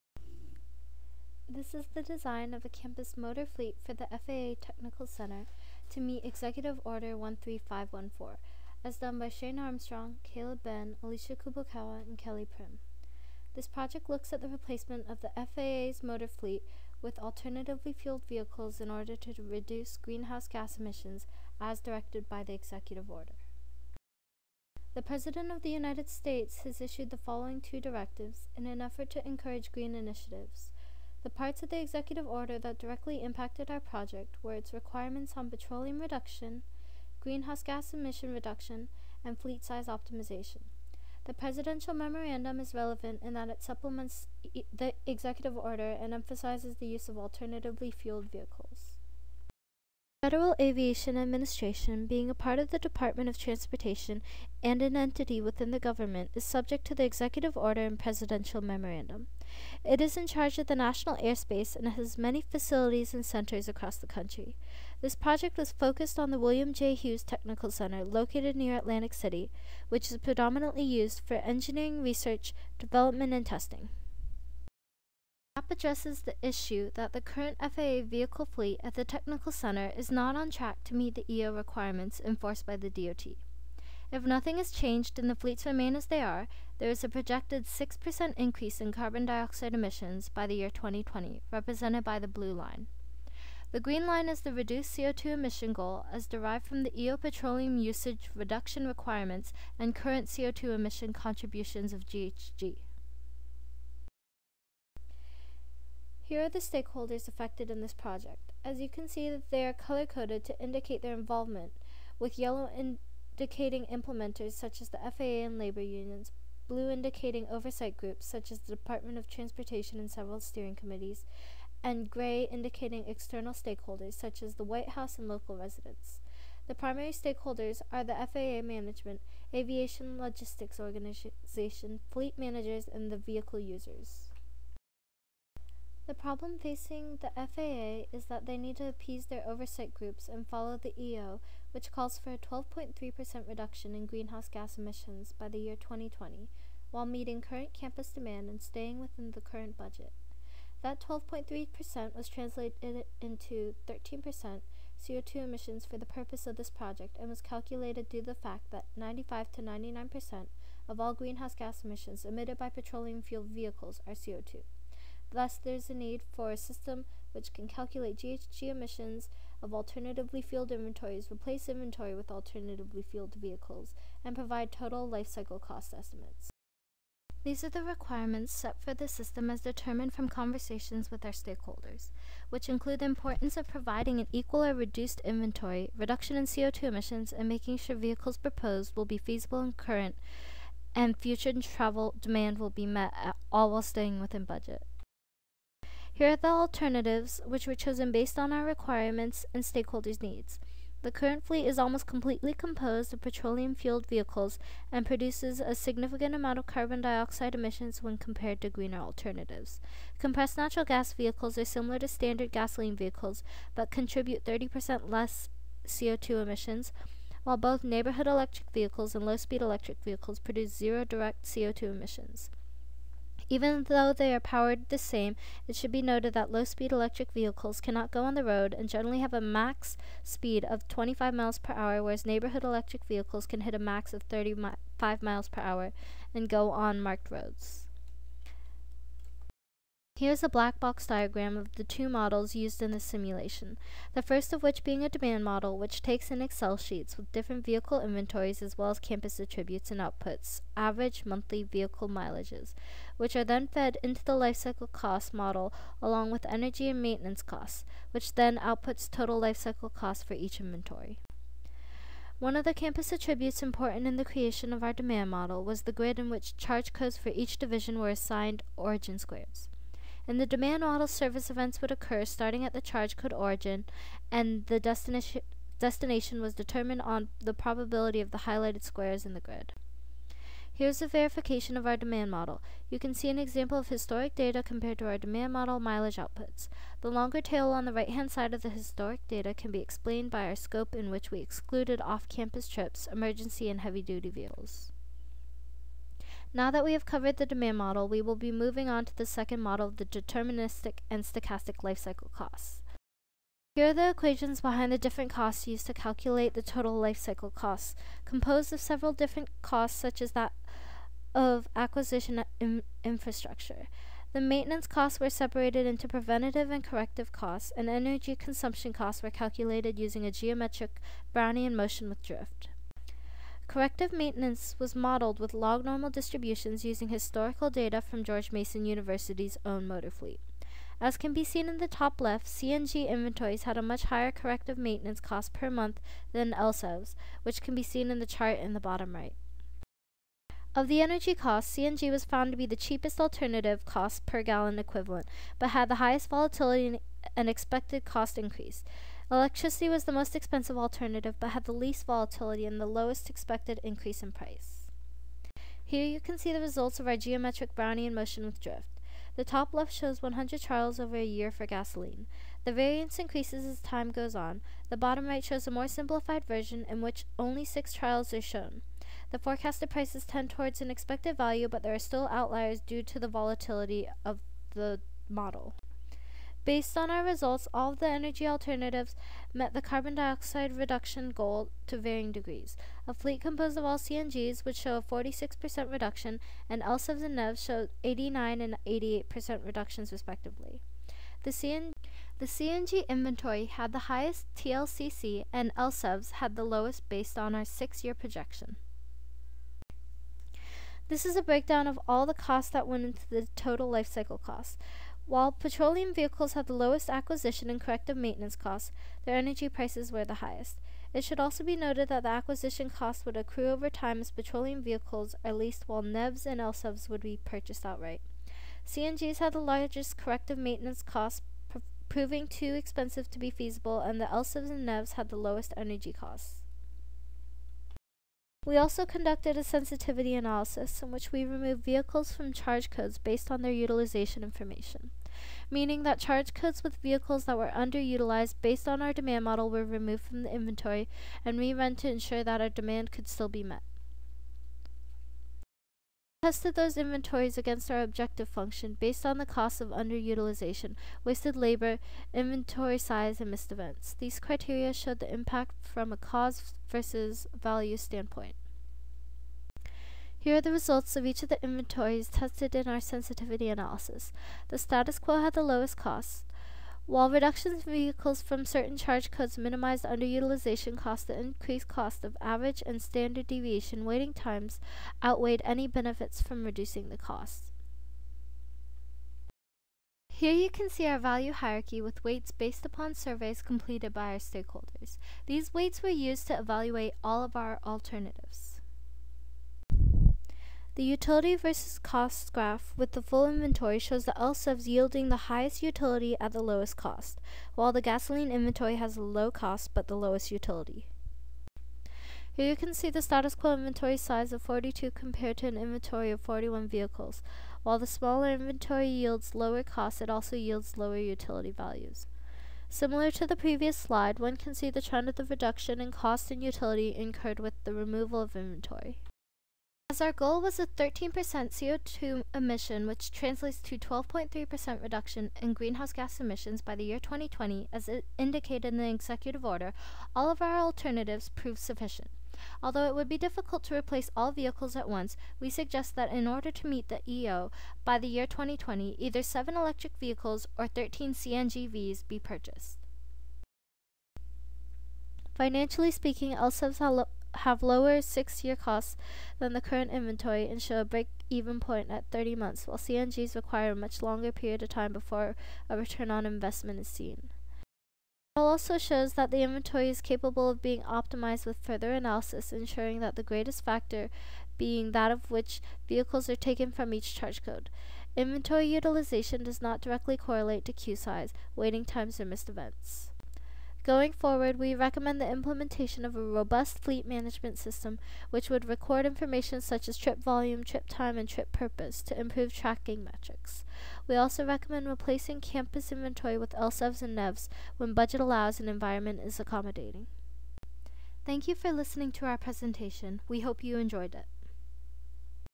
Campus Motor Fleet Analysis for the FAA Technical Center to Meet Executive Order 13514
George Mason University, Senior Design Capstone Project, "Campus Motor Fleet Analysis for the FAA Technical Center to Meet Executive Order 13514." Students: Shane Armstrong, Caleb Benn, Alicia Kubokawa, Kelly Prim